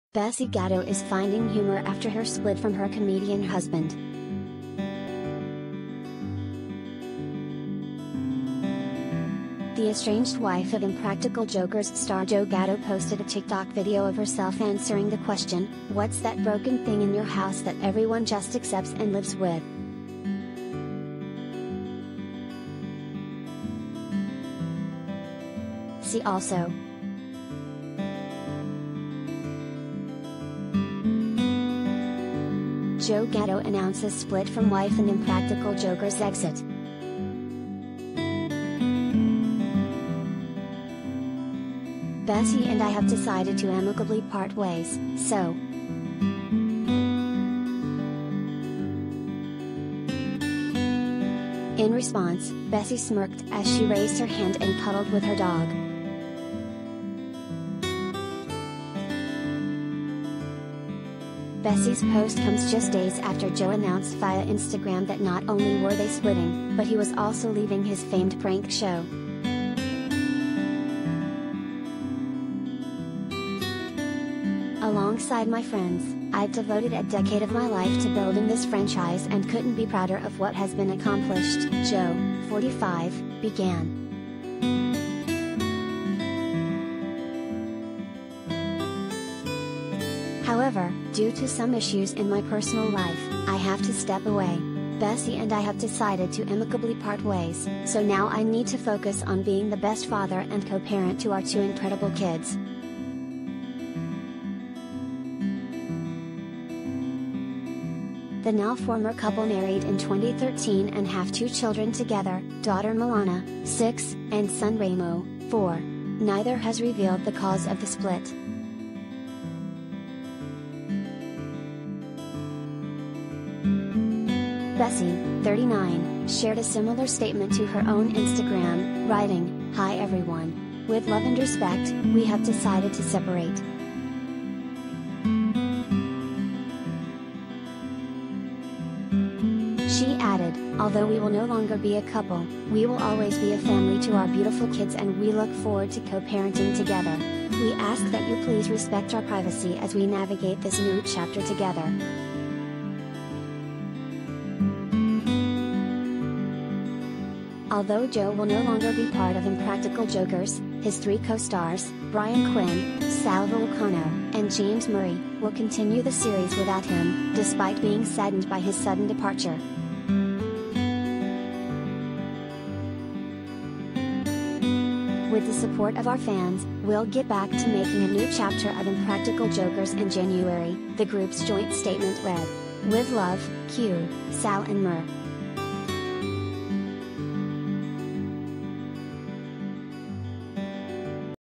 ‘Impractical Jokers’ star Joe Gatto’s estranged wife ‘broken’ after split
Bessy Gatto is finding humor after her split from her comedian husband.
The estranged wife of “Impractical Jokers” star Joe Gatto posted a TikTok video of herself answering the question, “What’s that broken thing in your house that everyone just accepts and lives with?”
see also
Joe Gatto announces split from wife and ‘Impractical Jokers’ exit
"Bessy and I have decided to amicably part ways, so...
In response, Bessy smirked as she raised her hand and cuddled with her dog.
Bessy’s post